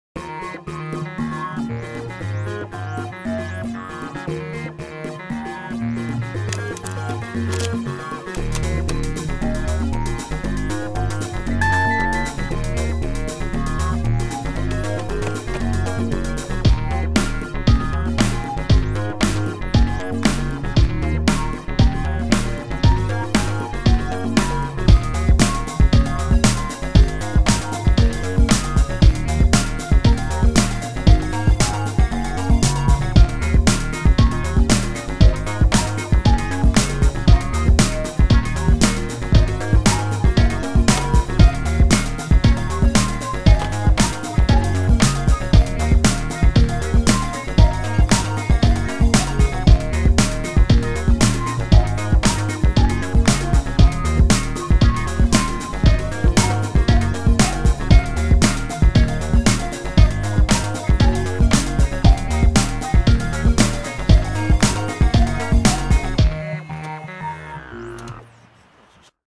PWS Introductie Elektrische auto
Korte introductiefilm voor een profielwerkstuk-presentatie over elektrische auto's. korte samenvatting van een middagje rijden met de mitsubishi I-Miev.